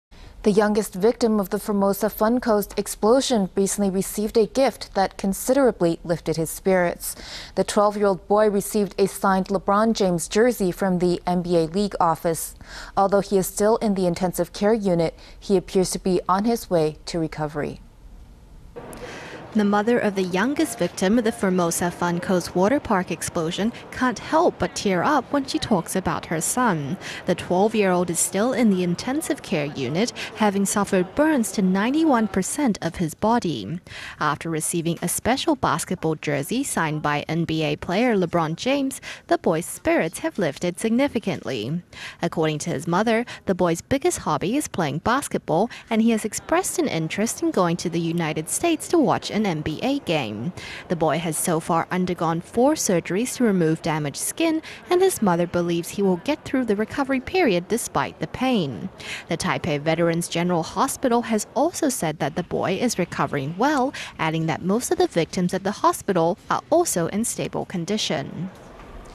小皇帝送球衣 昏沉李小弟恢復精神 LeBron James jersey lifts spirits of 12-year-old explosion victim—宏觀英語新聞
The youngest victim of the Formosa Fun Coast explosion recently received a gift that considerably lifted his spirits. The 12-year-old boy received a signed LeBron James jersey from the NBA League Office. Although he is still in the intensive care unit, he appears to be on his way to recovery.

八仙塵爆意外中，年紀最小的12歲李小弟，目前仍在台北榮總加護病房治療中，而為了鼓勵他對抗病魔，日前美國NBA官方、特地送上騎士隊當家球星小皇帝詹姆士的親筆簽名球衣，讓原本昏沉沉的李小弟，眼睛睜得好大，精神都恢復了。